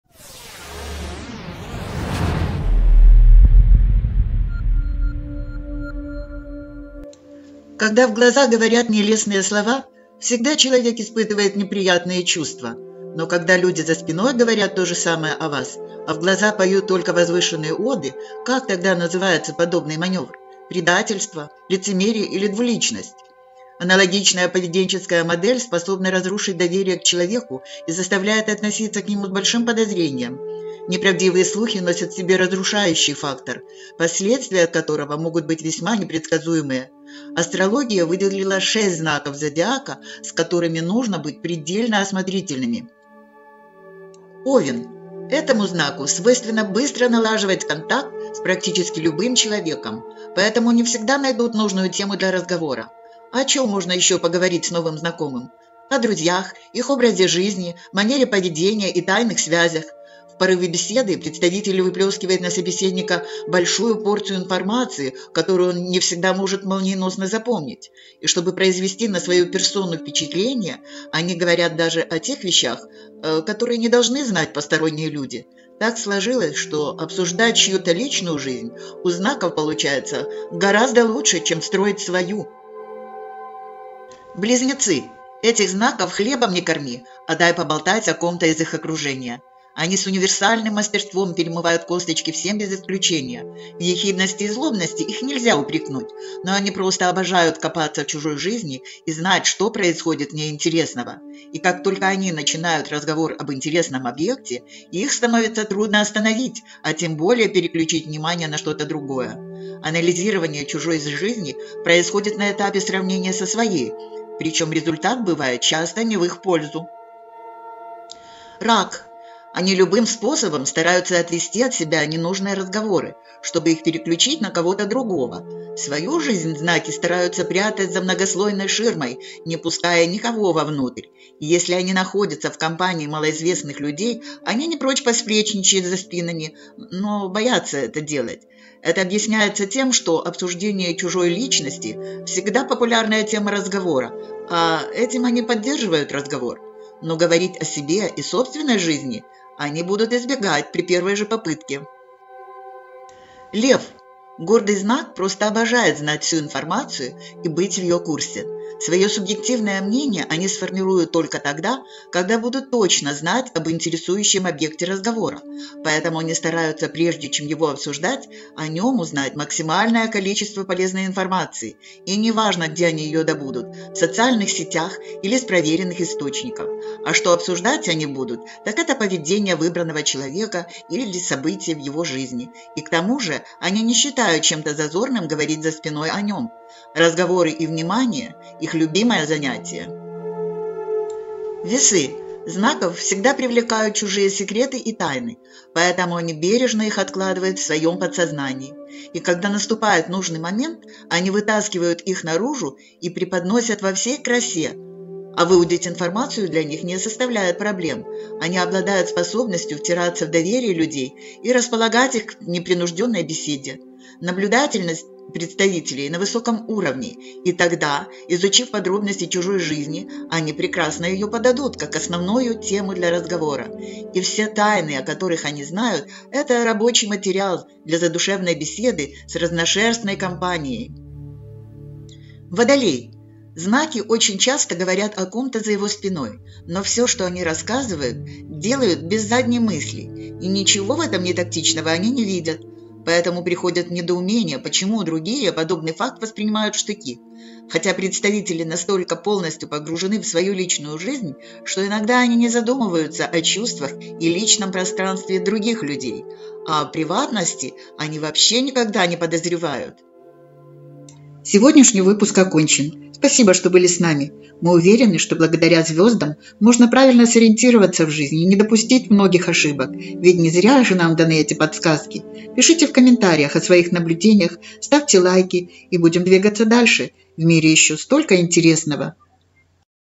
САМЫЕ ДВУЛИЧНЫЕ ЗНАКИ ЗОДИАКА
Рейтинг самых двуличных представителей звездного небосклона, которые способны на сплетни за спиной.

Когда в глаза говорят нелестные слова, всегда человек испытывает неприятные чувства. Но когда люди за спиной говорят то же самое о вас, а в глаза поют только возвышенные оды, как тогда называется подобный маневр? Представительство, лицемерие или двуличность? Аналогичная поведенческая модель способна разрушить доверие к человеку и заставляет относиться к нему с большим подозрением. Не правдивые слухи носят в себе разрушающий фактор, последствия от которого могут быть весьма непредсказуемые. Астрология выделила 6 знаков Зодиака, с которыми нужно быть предельно осмотрительными.